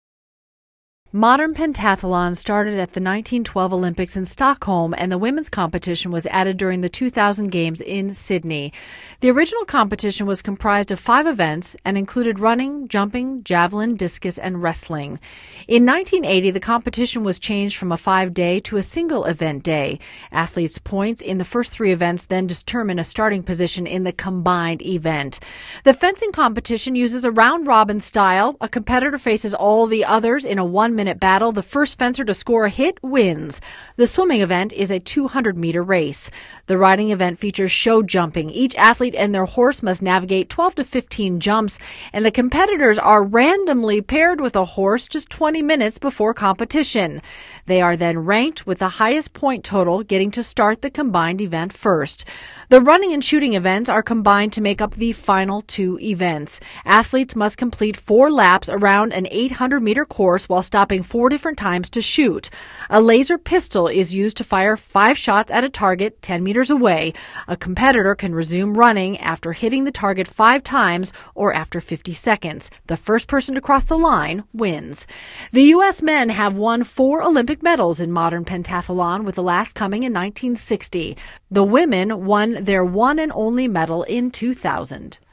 Modern Pentathlon
This is an animation I created for the Olympics event "Modern Pentathlon"

Side note: Early script- 1980 year is later changed to correctly reflect the true year

Script written by David Hunt, voiced by Cheryl Preheim